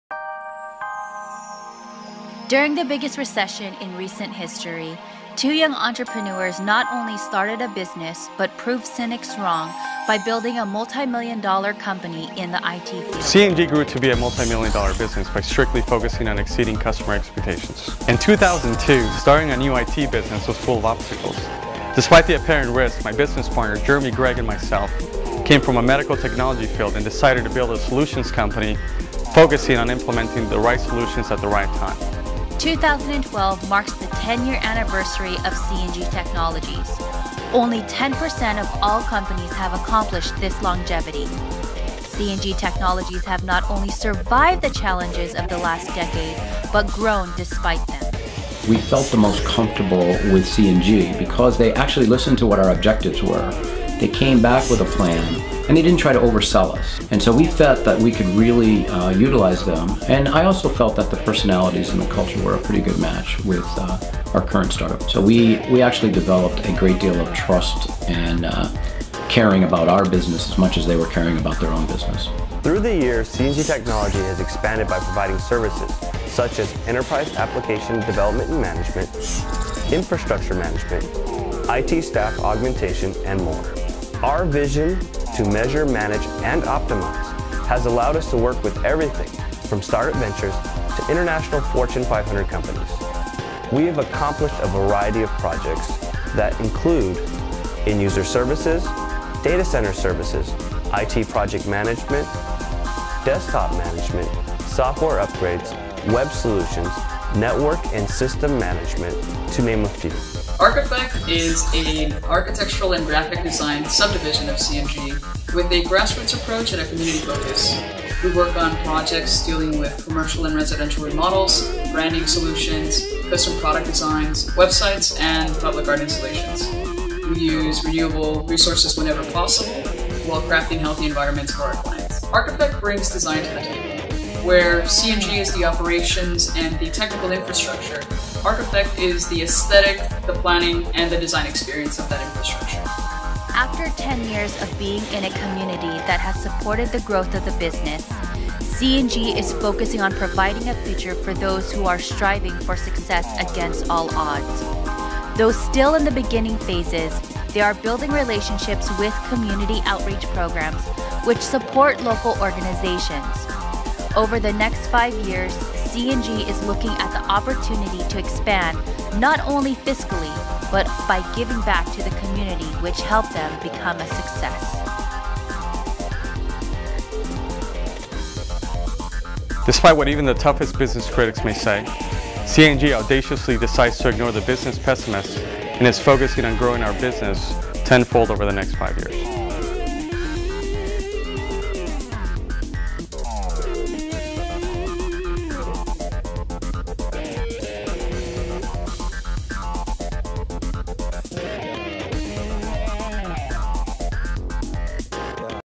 C&G Technologies-10 Years
2012 marks the ten year anniversary of C & G Technologies. C & G's passion to help companies and logistic operations accelerate their growth through the deployment of innovative information technology solutions that increase clients'
strategic and operational effectiveness. A philosophy deeply ingrained into C & G's working culture that ultimately acts as the foundation for their unparalleled customer service.